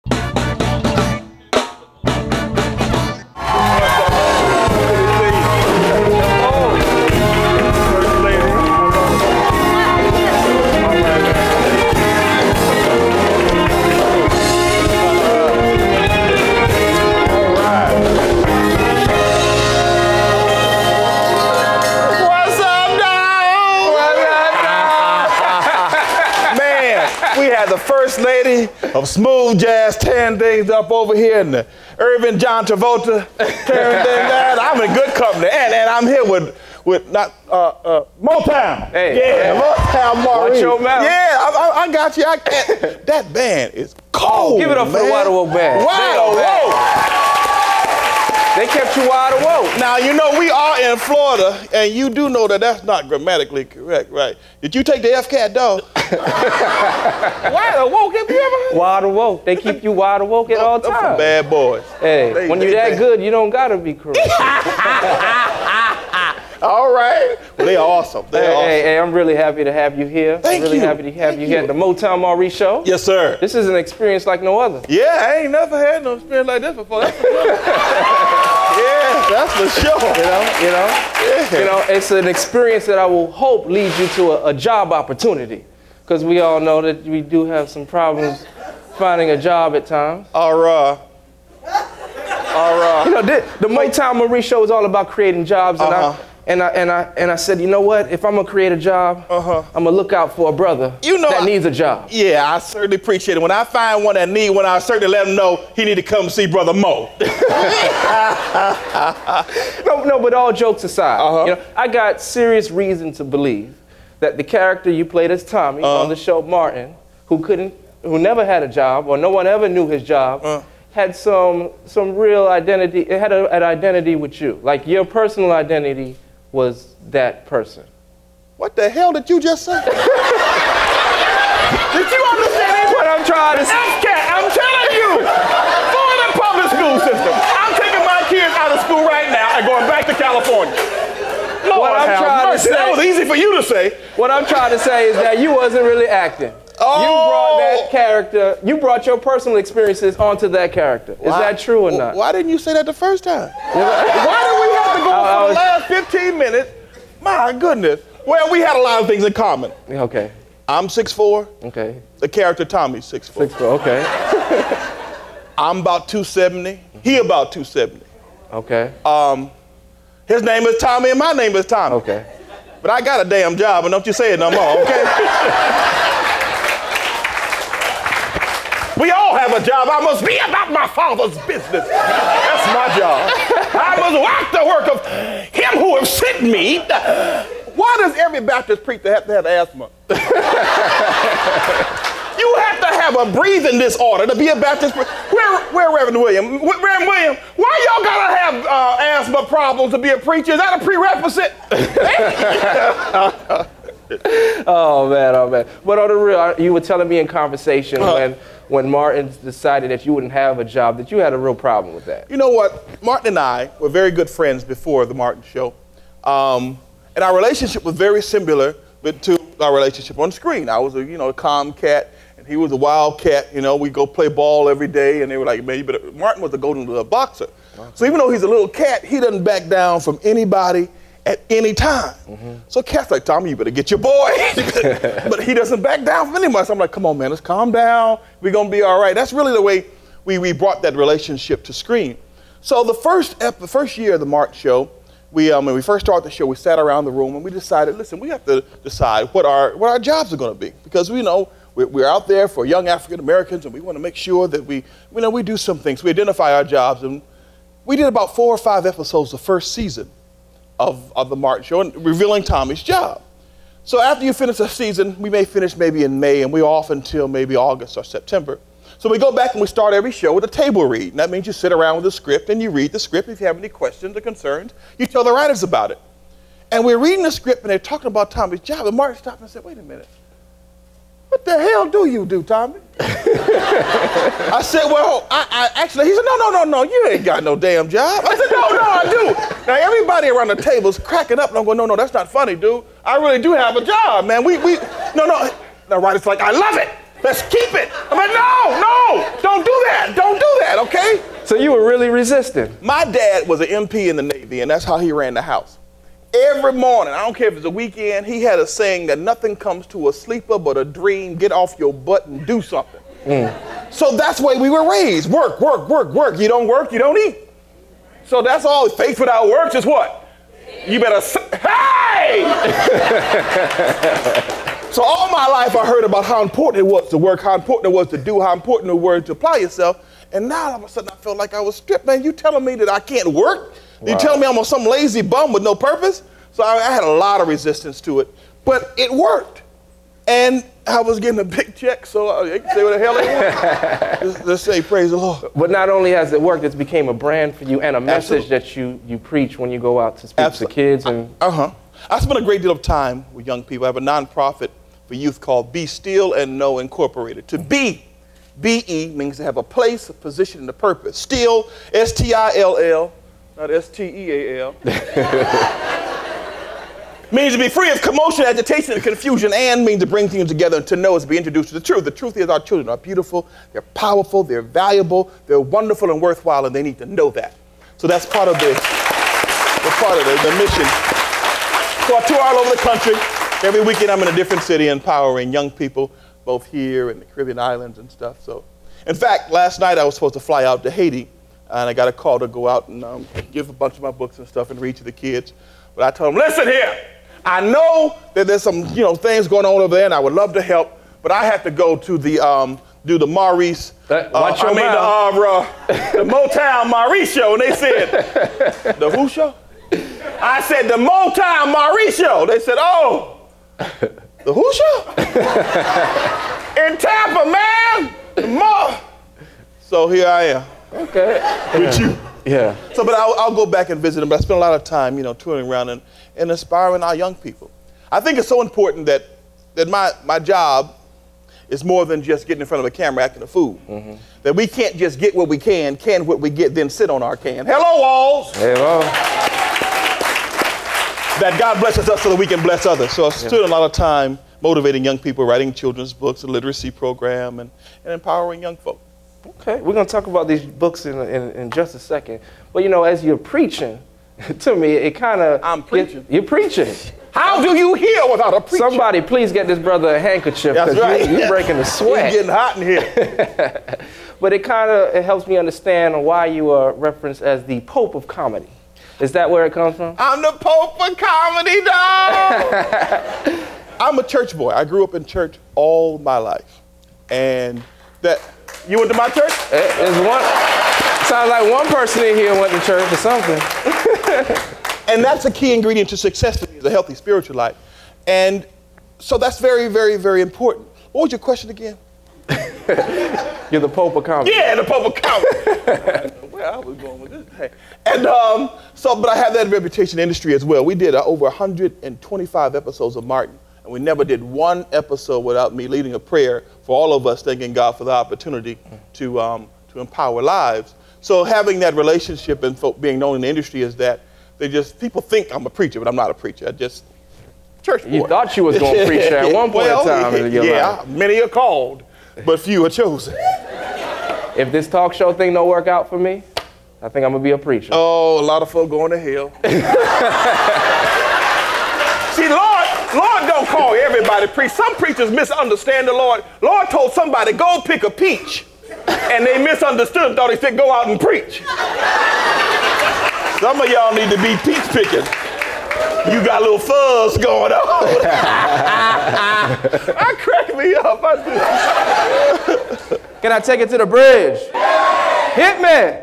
Motown Maurice - Tommy Ford Part 1
Motown Maurice interviews Tommy Mikal Ford, best known for his role as Tommy on the hit FOX television comedy, MARTIN.